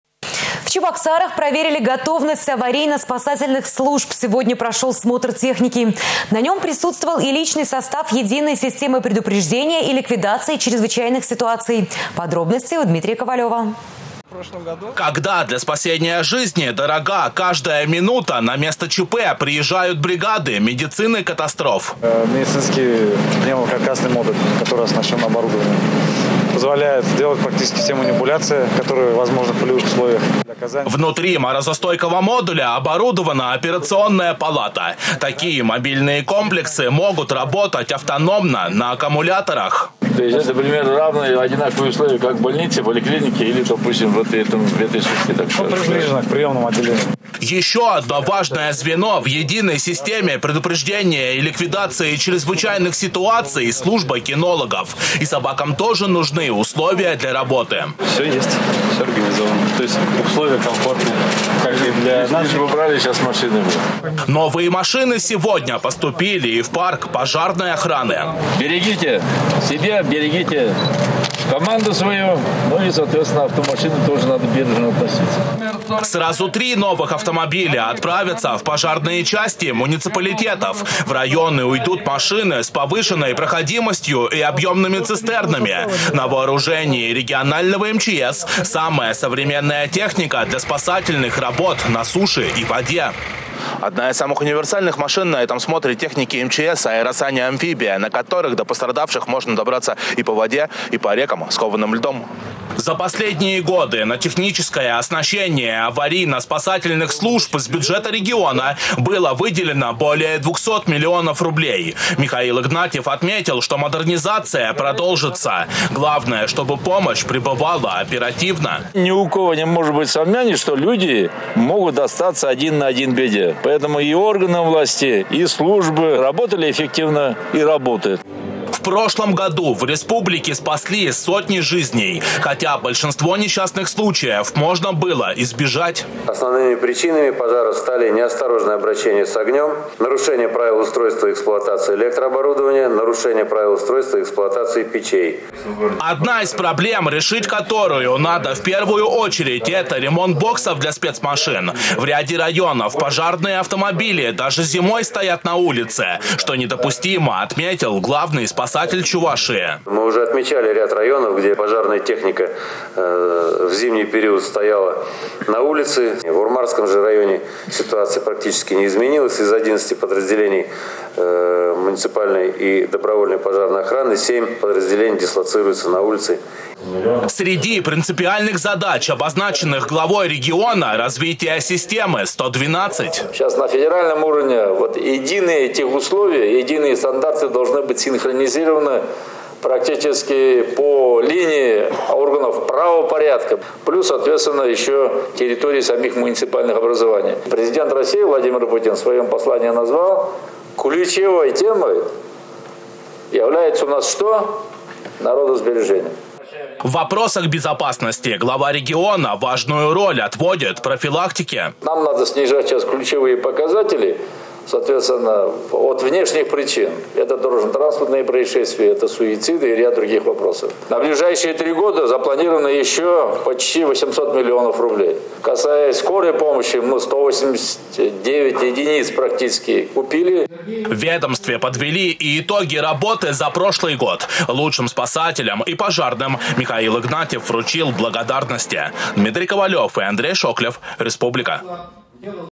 Смотр техники МЧС
В Чебоксарах проверили готовность аварийно-спасательных служб. Сегодня прошел смотр техники. На нем присутствовал и личный состав единой системы предупреждения и ликвидации чрезвычайных ситуаций. Подробности у Дмитрия Ковалёва.